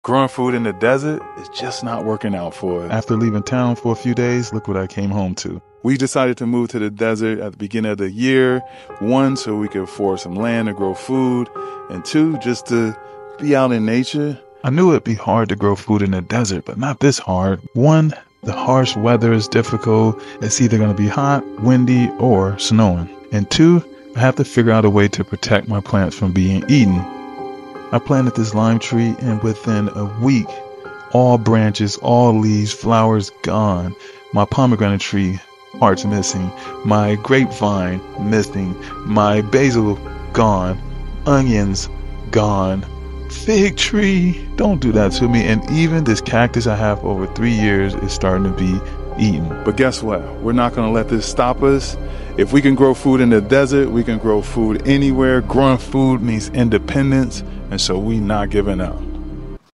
Growing Food in the desert 🏜️ might not be a good idea 💡
Don’t Let Anything Stand In The Way Of Your Dreams!

 Growing Food In The Desert Part 1

This year, we moved to the desert to find affordable land and more peace in nature. Over the last few years, we have spent more time camping in nature than in the city, so it was a great move. Growing food in the desert would be challenging, but we are now encountering issues we did not expect.

We knew the weather would be a concern, but we didn’t expect the wind and our plants to eat at such a fast rate. We see coyotes, rabbits, squirrels, chipmunks, desert wood rats, spiders, and insects. This does not bother me at all because I am in their space. After all this, our best hope is to build a cheap and affordable greenhouse that can withstand the wind and keep the animals out.

So, if you have any tips on building a greenhouse, please send them to me! We are going to get this done. HELP! haha